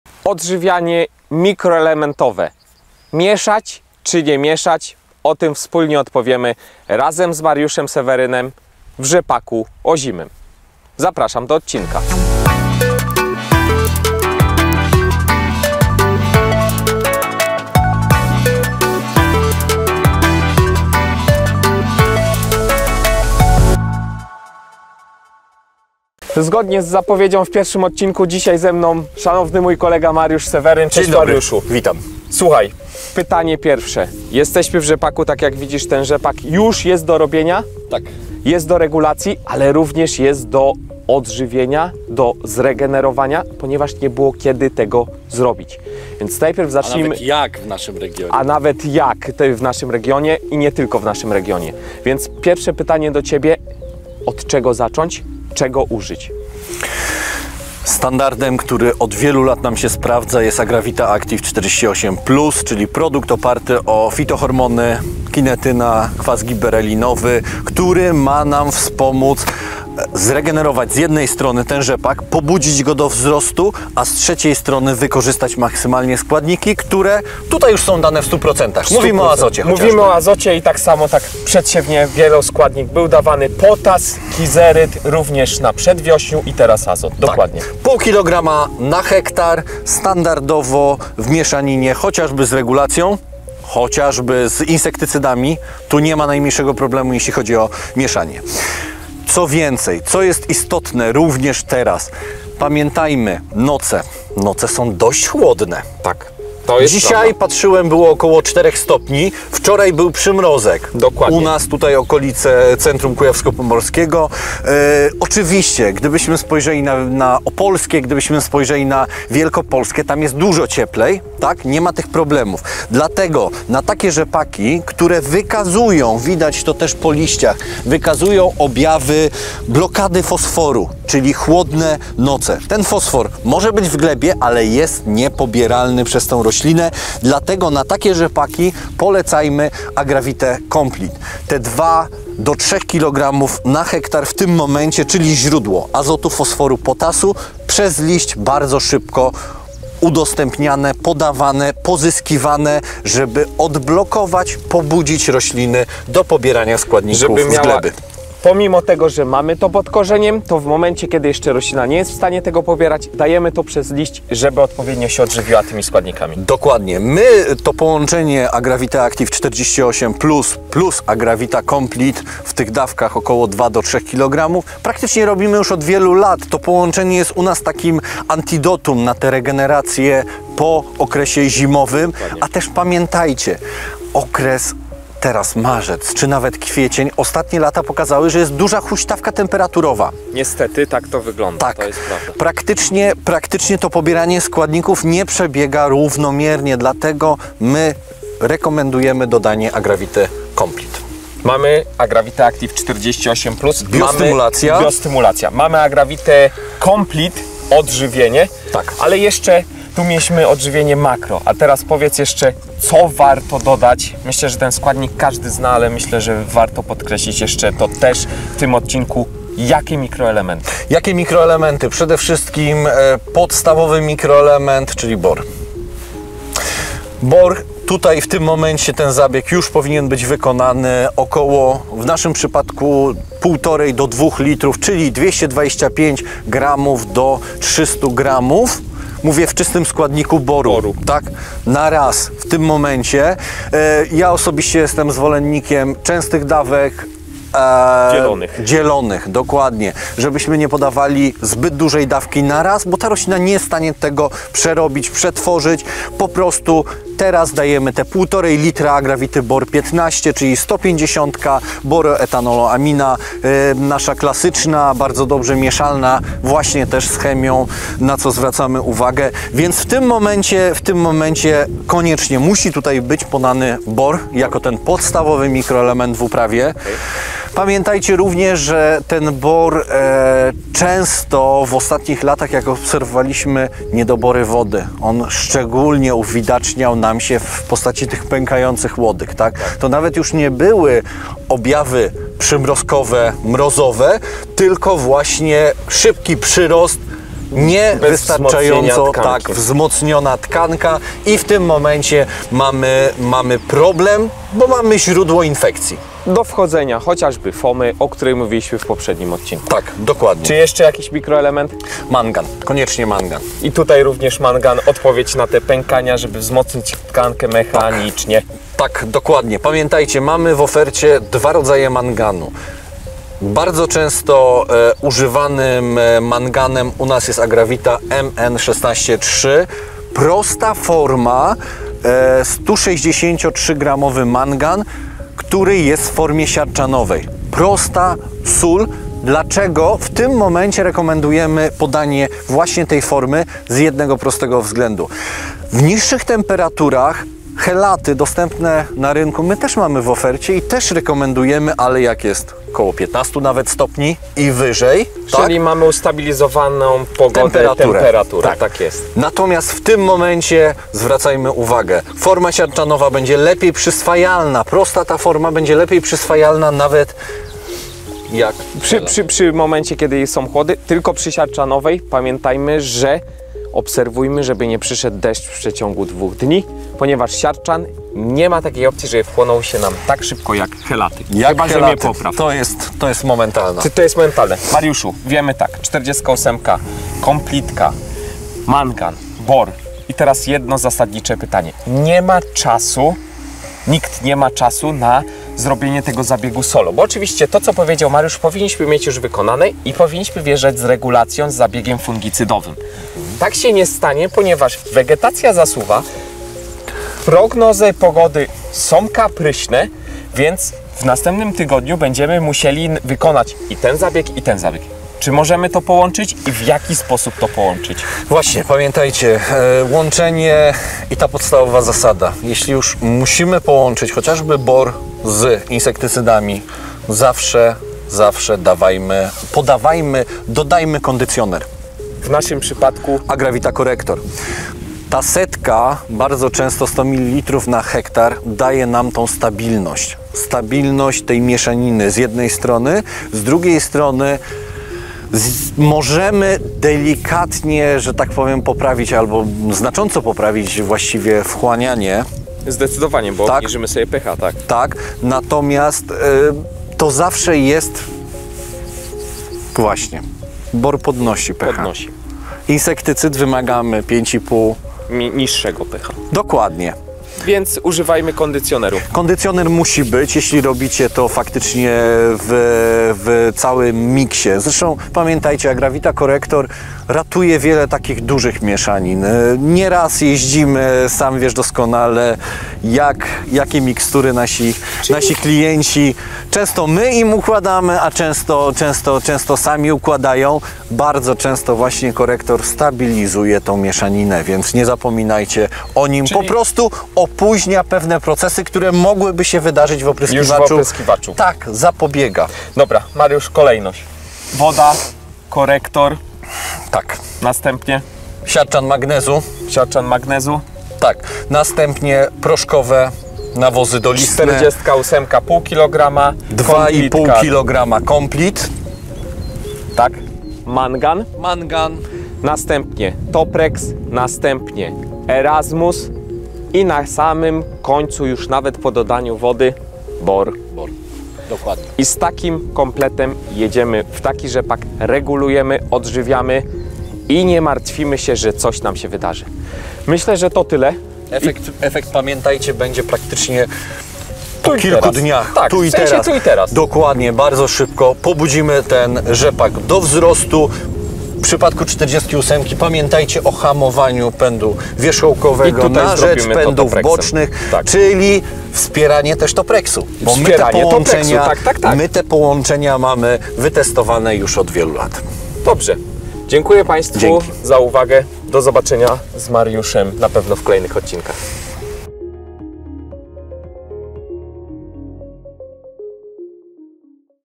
Odżywianie rzepaku. Mieszać czy nie mieszać?
Nadszedł czas na wiosenne odżywienie rzepaku w niezbędne mikroelementy. Wraz z naszym ekspertem, Mariuszem Sewerynem, wyjaśniamy jakich produktów użyć do wykonania zabiegu.

Rekomendujemy wykorzystanie preparatów: Agravita® Aktiv 48 Plus, Agravita® Complete, Agravita® Bór 15, Agravita® Mn 16,3 oraz Agravita® Korektor.